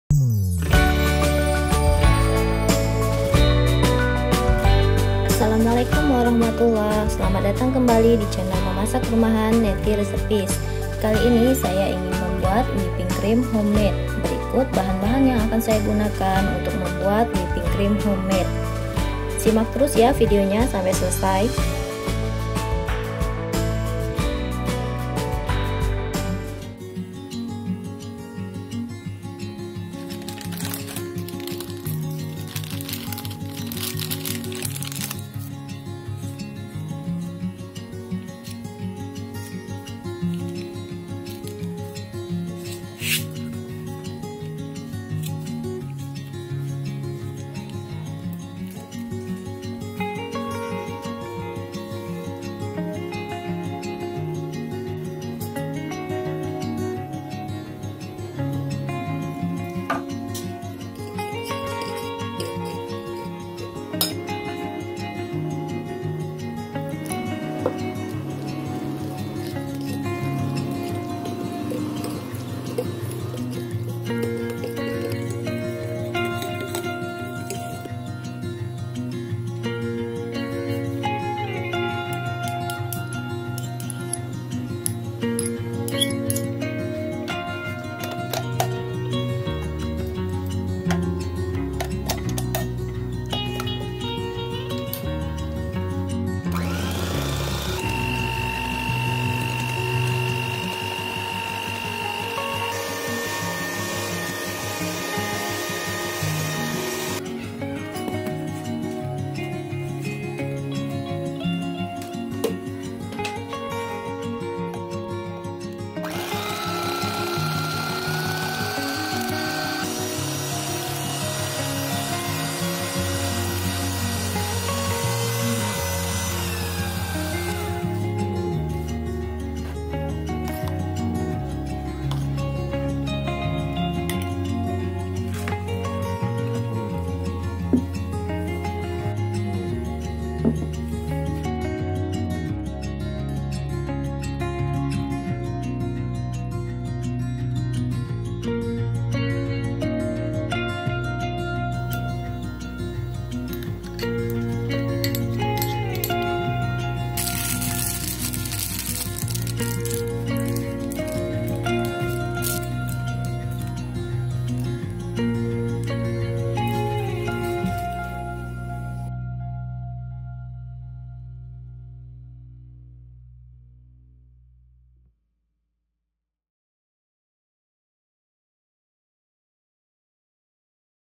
CARA MEMBUAT WHIPPING CREAM HOMEMADE UNTUK DESSERT BOX CAKE
Kali ini saya membuat Whipping Cream Homemade, untuk lapisan dessert box yang akan saya buat di video selanjutnya.

Whipping cream homemade Untuk rasa, whipping cream homemade ini sama-sama creamy dan lembut persis seperti whipping cream instan.

Dan menurut saya, whipping cream homemade ini hanya cocok untuk minuman atau dessert box. Kurang cocok kalau di aplikasikan sebagai pelapis cake.

- 50 ml air es ( harus benar-benar dingin )
- 1 sendok makan SP
- sejumput garam

Please Enjoy to watch this video, see you in next video recipe.